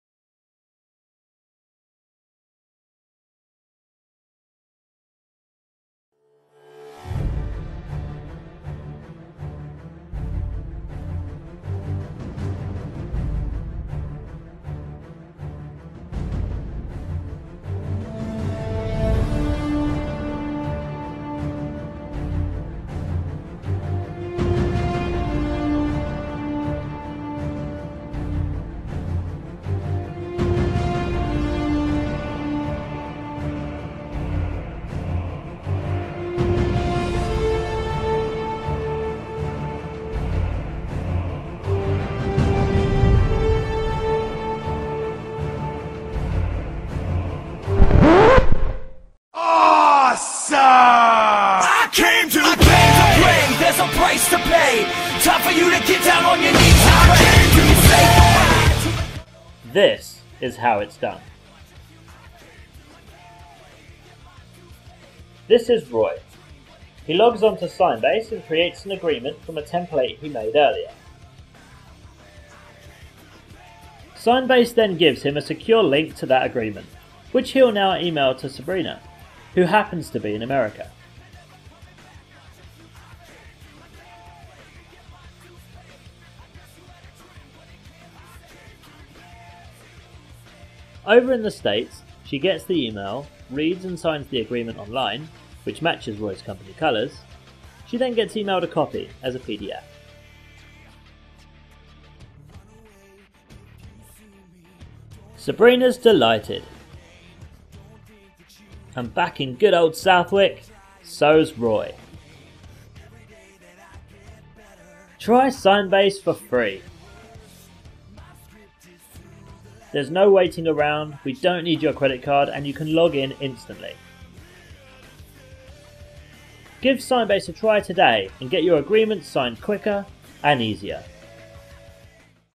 Official SignBase Trailer
SignBase is a simple online document signing app for small business owners and sales professionals. This trailer walks us through old way of getting documents signed, and the new way. The awesome way. Sign up for a free account at GetSignBase.com